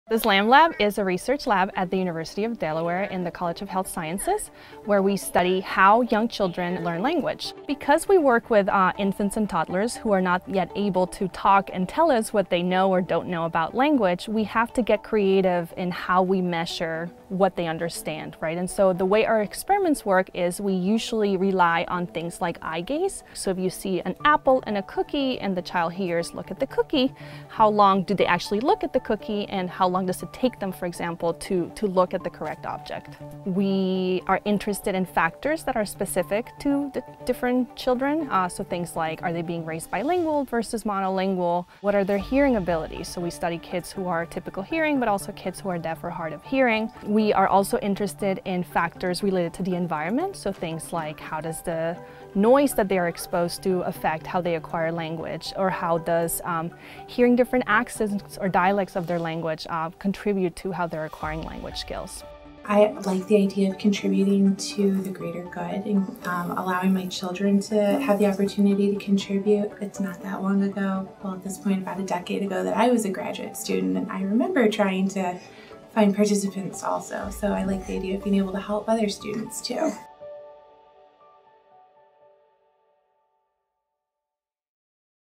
Speech Language Acquisition & Multilingualism Lab at UD
The Speech Language Acquisition & Multilingualism (SLAM)
Lab is part of the Communication Sciences and Disorders program in the
College of Health Sciences at the University of Delaware.  The lab's
research explores a number of questions related to how children learn
language:

- What factors make learning words easier?
- What type of information facilitates word recognition?
- How do children process different dialects and accents of their language?
- Do bilingual children acquire language skills differently?